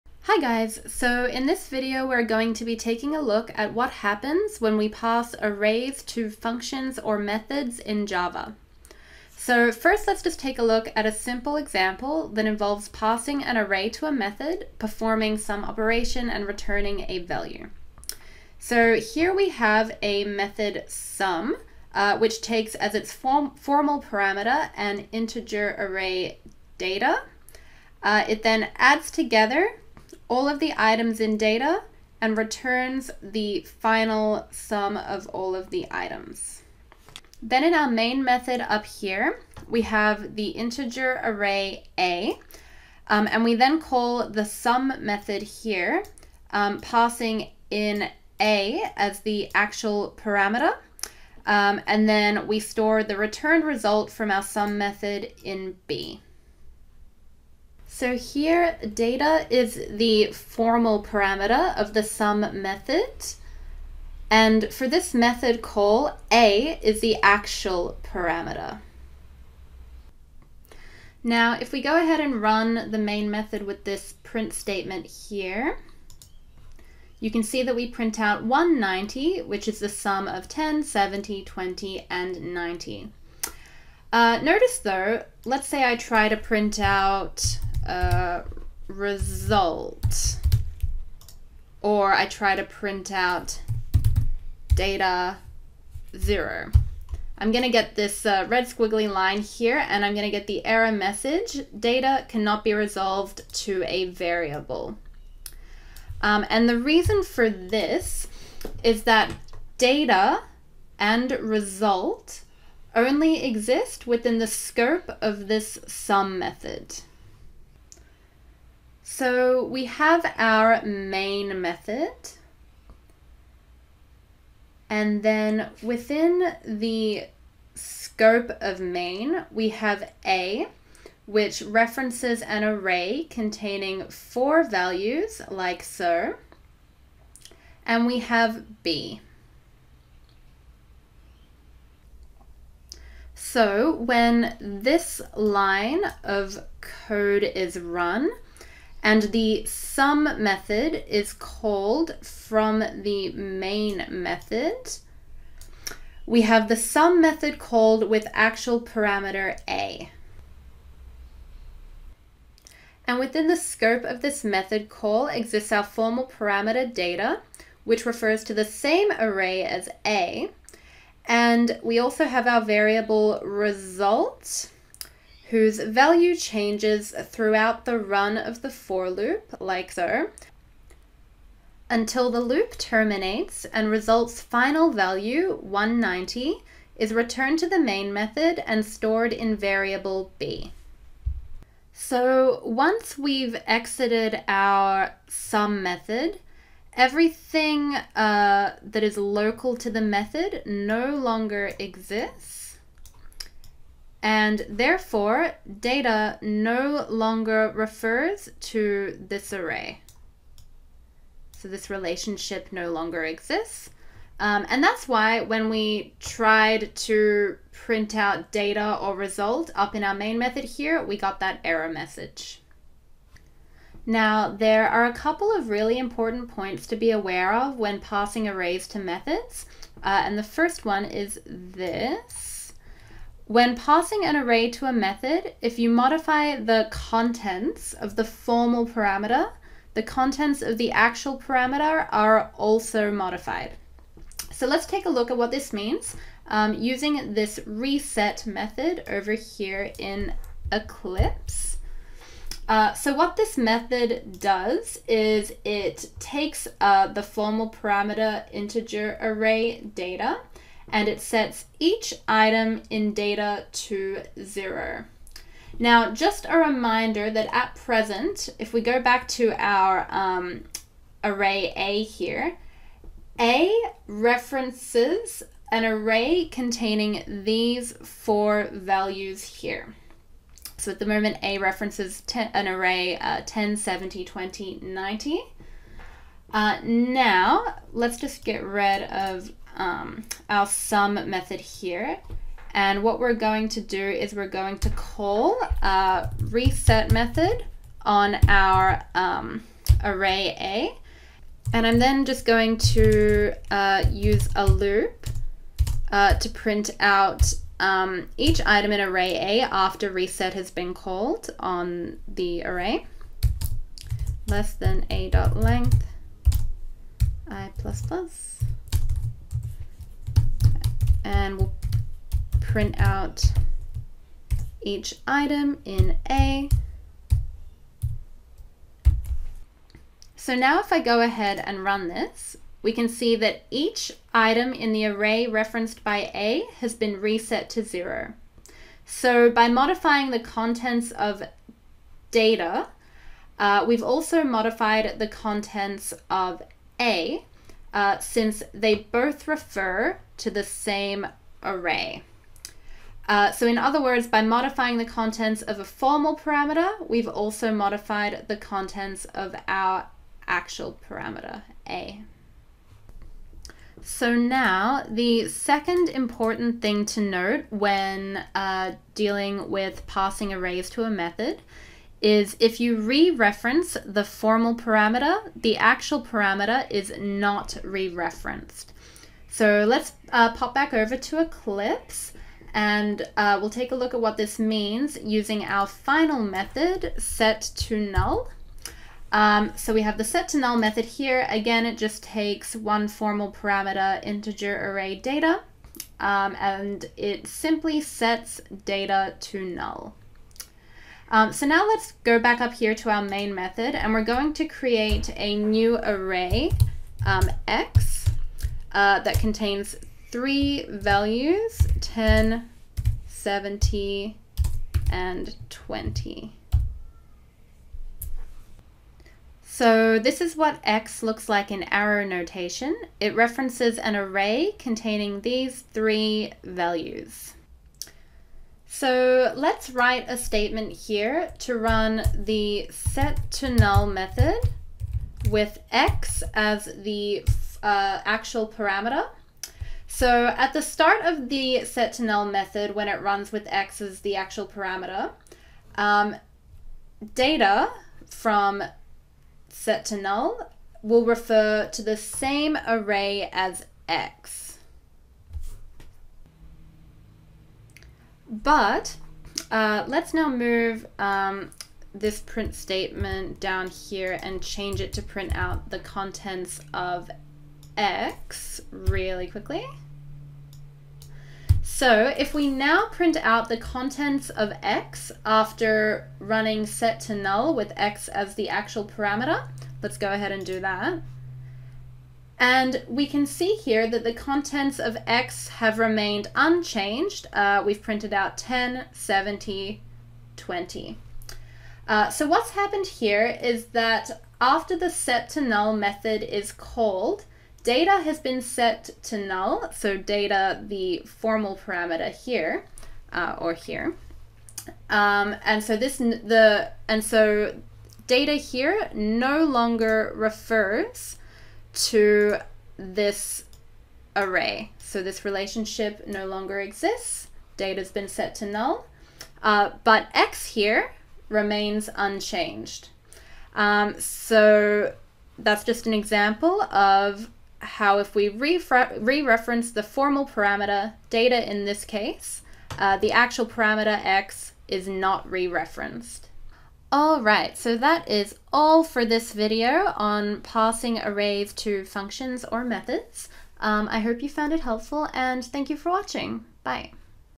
Passing Arrays to Functions or Methods
In this video we'll be taking a look at what happens when we pass arrays to functions or methods in Java.